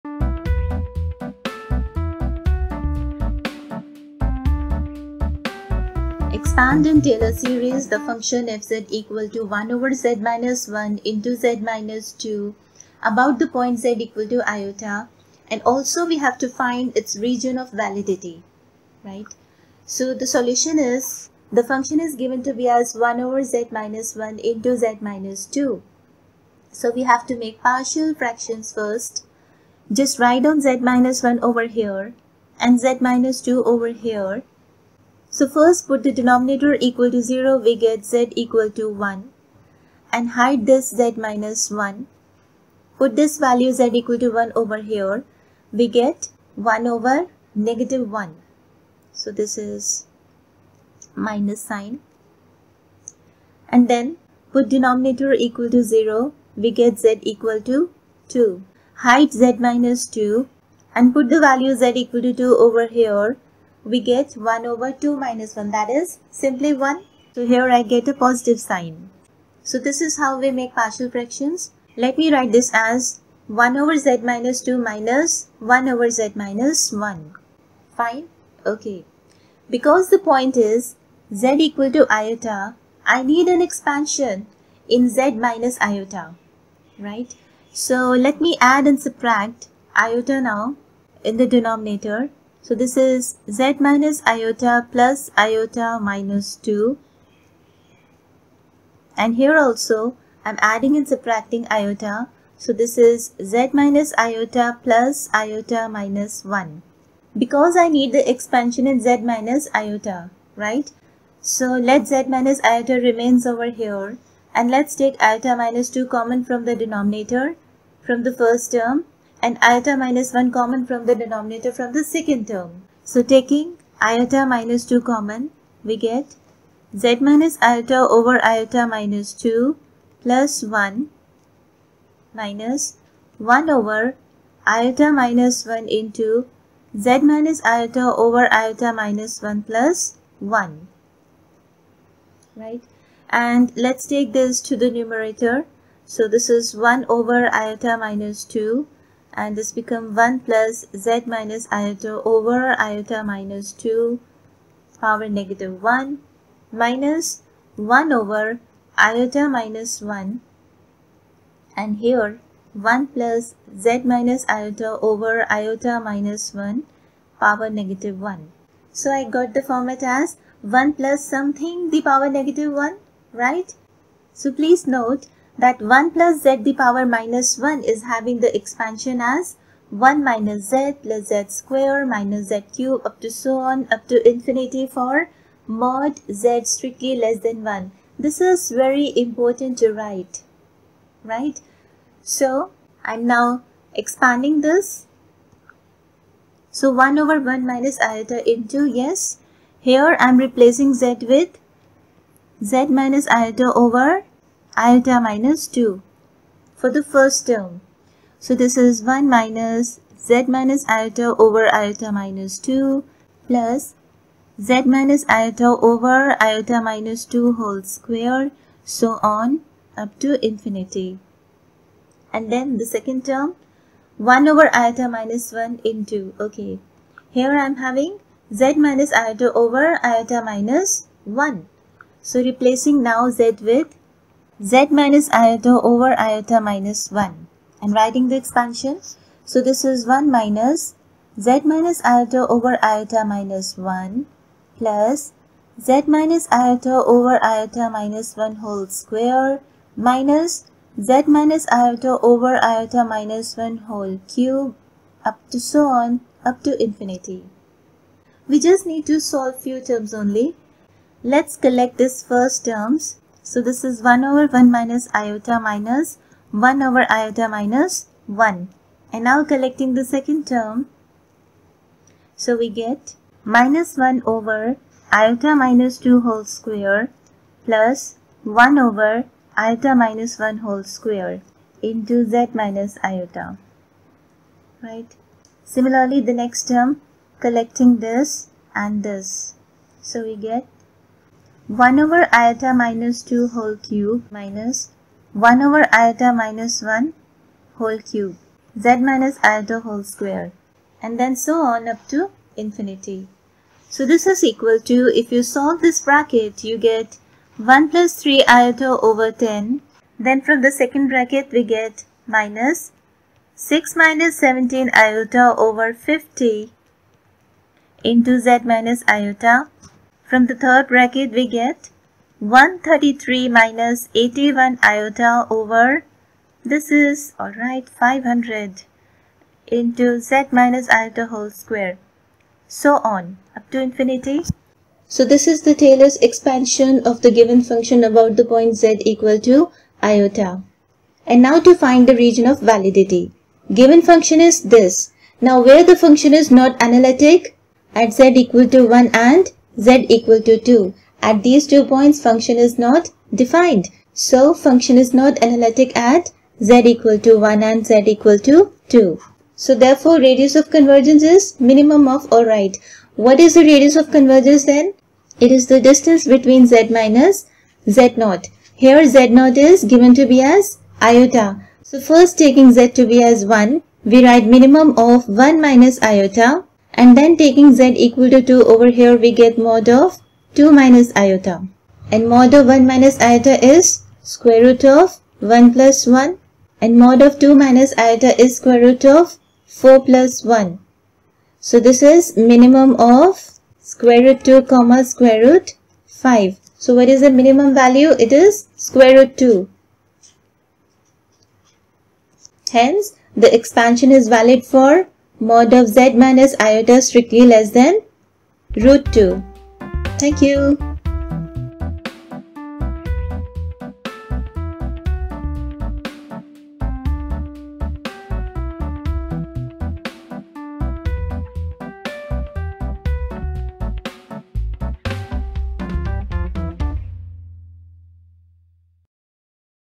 Complex Analysis for csir net | Taylor's series Expansion of a function f(z) = 1/(z-1)(z-2)
Complex Analysis for csir net
Complex Analysis

Taylor's Series Expansion about z = i